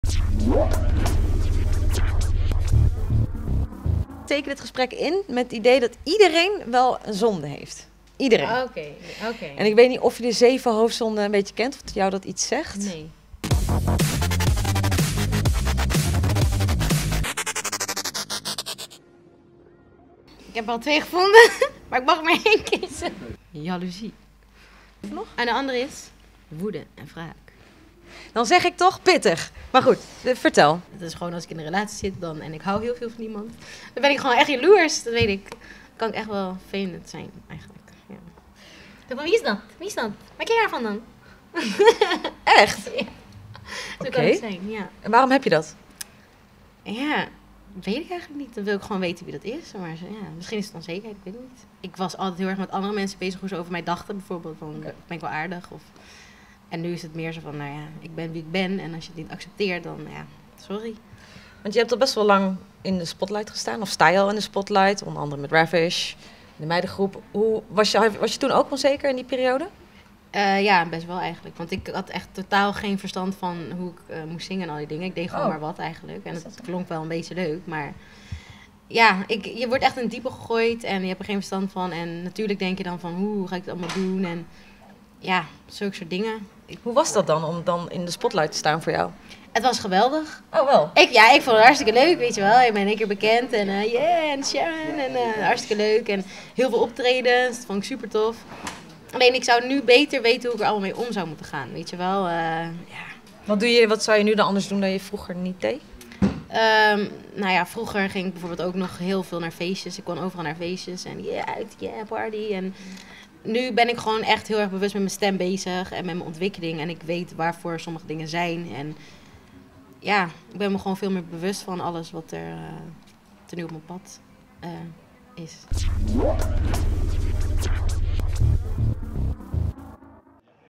Zeven Hoofdzonden: Sharon Doorson
Ook Sharon Doorson is onzeker! Kan jij je dit voorstellen? Ze legt het voor jullie uit!

Volg The V Reporter ook online!